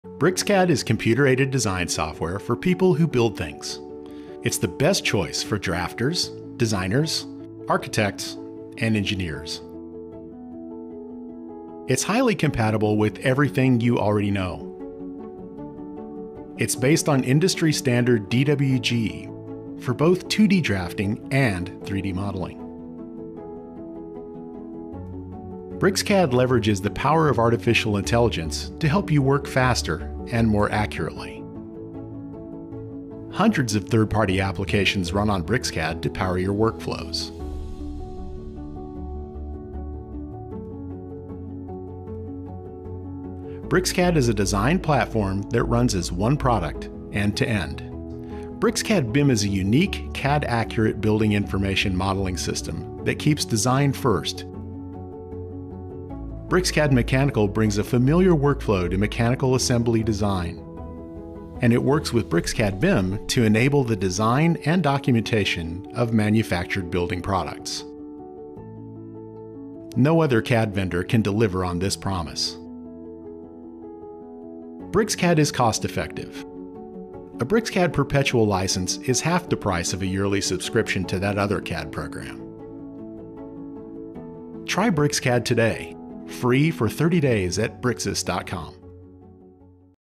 O que é o BricsCAD
BricsCAD: um conjunto de ferramentas profissionais de CAD e modelagem para projetistas, inovadores e engenheiros. Sem concessões.
Uma Plataforma de Projeto para todas as especialidades.
Todos os níveis de produto do BricsCAD são criados em uma plataforma única de projeto, que oferece seis conjuntos de ferramentas funcionais.